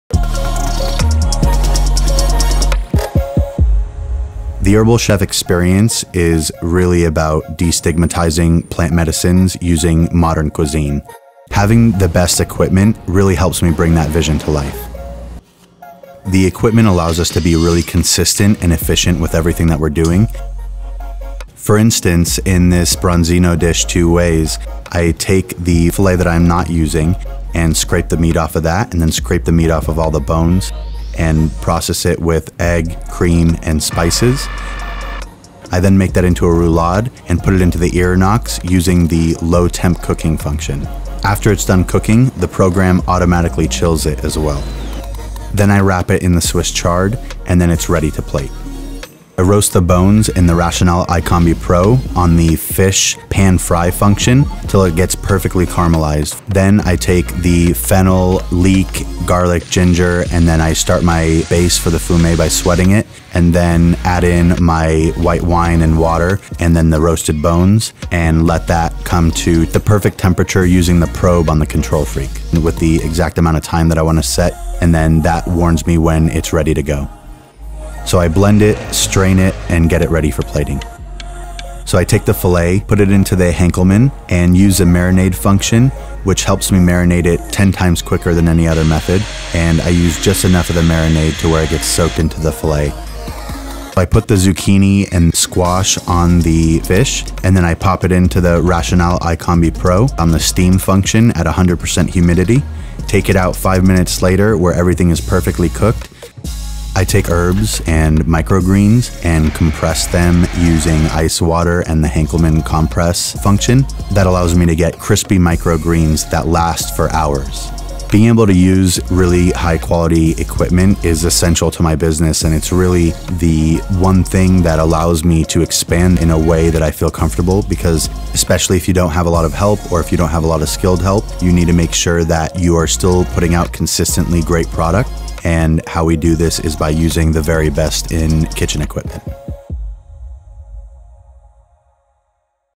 Branzino 3 Ways - Crispy Bone Fume | Forcemeat Roulade | Steamed Fillet
Chef Chris Sayegh shows how he makes a dish using branzino 3 ways by steaming the fillet, making a roulade from the scraps and roasting the bones to make a fume. All this is accomplished by using the latest kitchen technology.